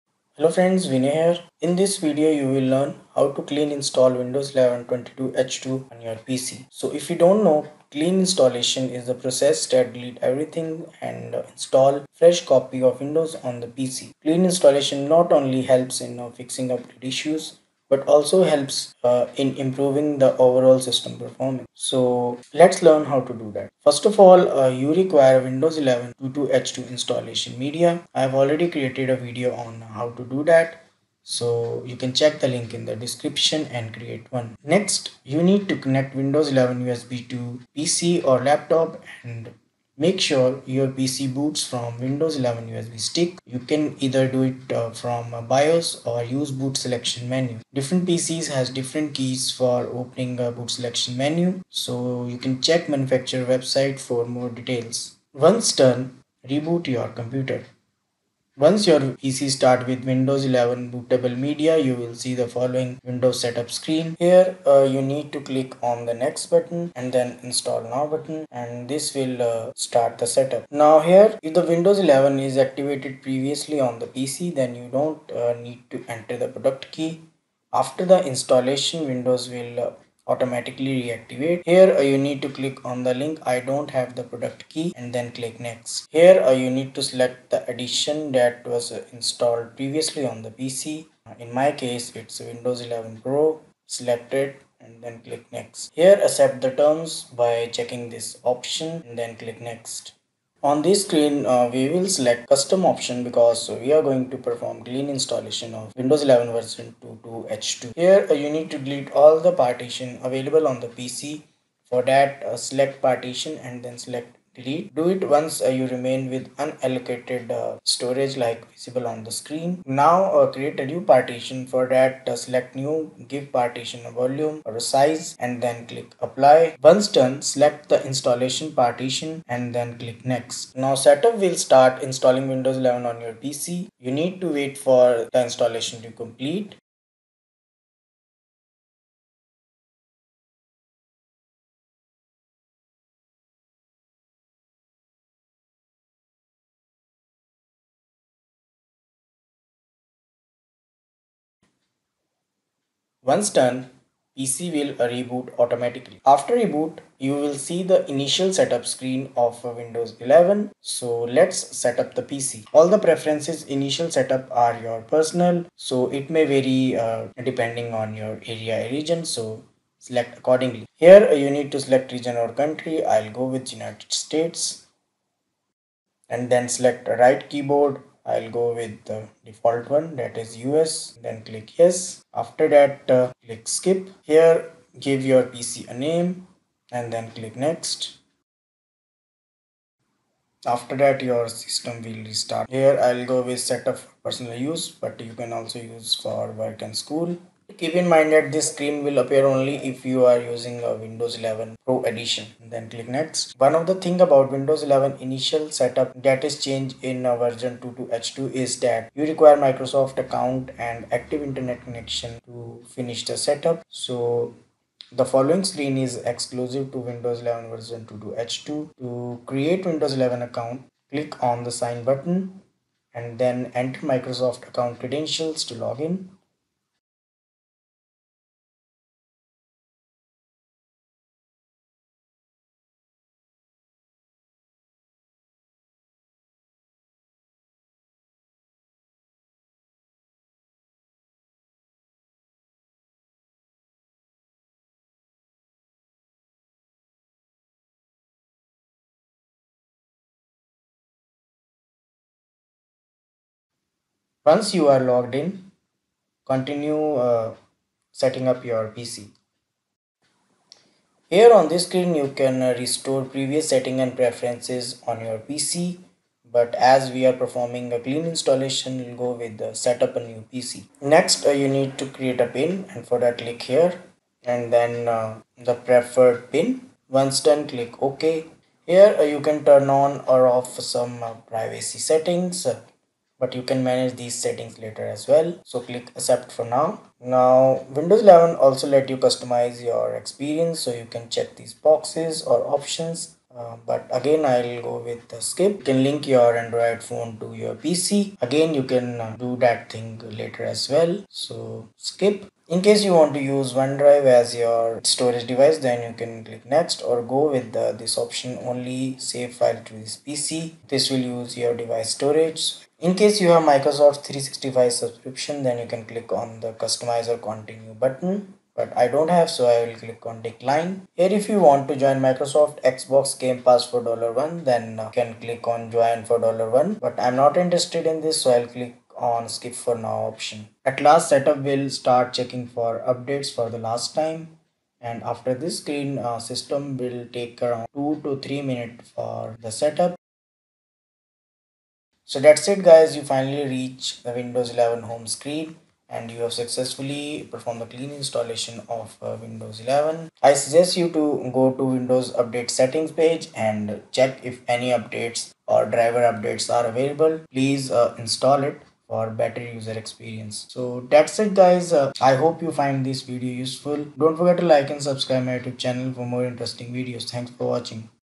How to Clean Install Windows 11 22H2 on New PC | Laptop | New SSD
In this video, you will learn how to perform a clean installation of Windows 11 22H2 2022 Update on a New PC or Laptop from USB